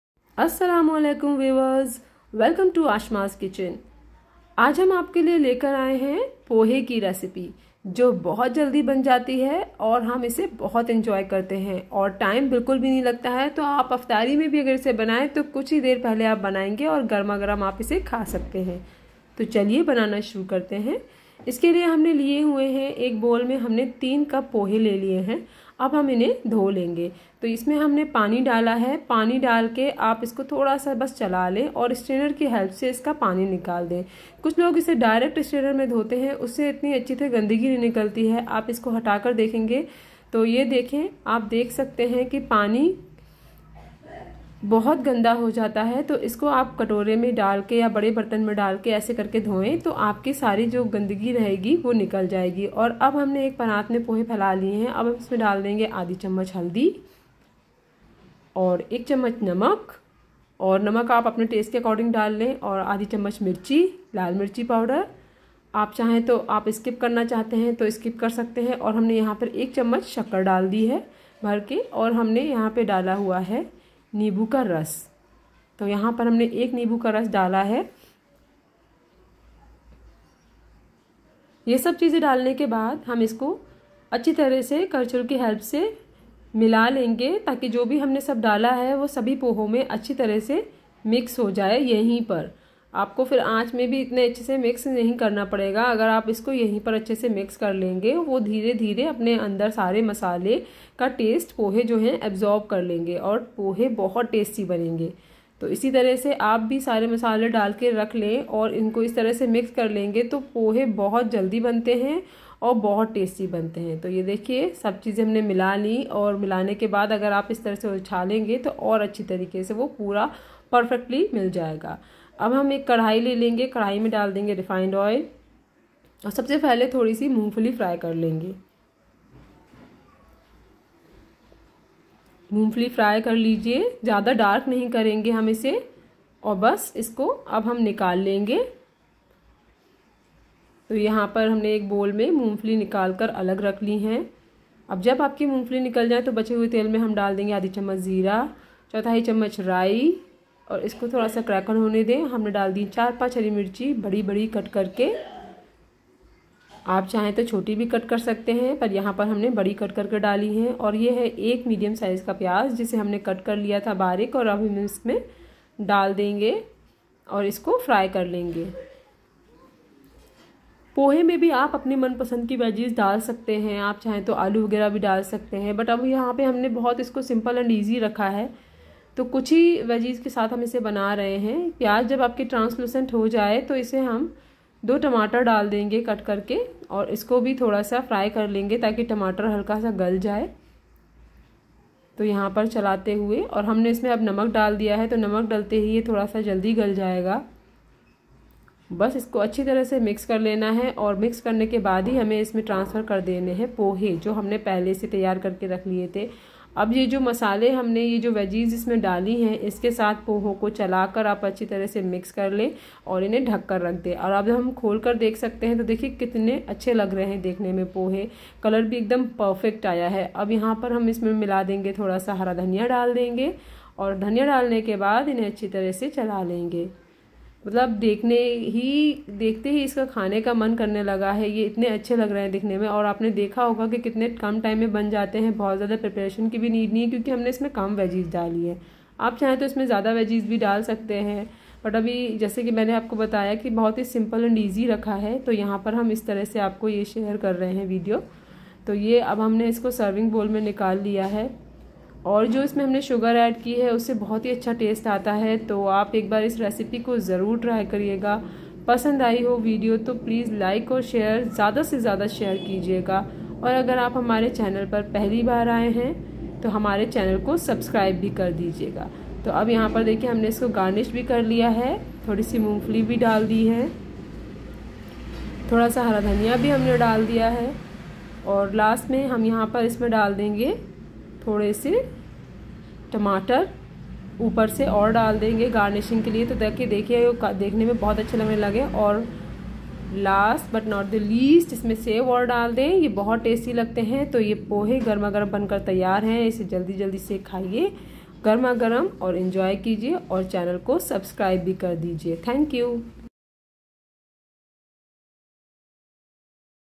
पोहा तो बहुत खाया होगा पर इस तरह से नहीं बनाया होगा | How to make poha | poha recipe | in hindi
In this video, we are going to show you how to make Desi Aam ka Achar in Bundelkhanid style. We hope, you'll like it.
Is Video mein hum aap ko dikhane ja rahe hain ki Bundelkhandi Desi Aam ka Achar kaise banayein.. Ummeed karte hain ki apko pasand ayga..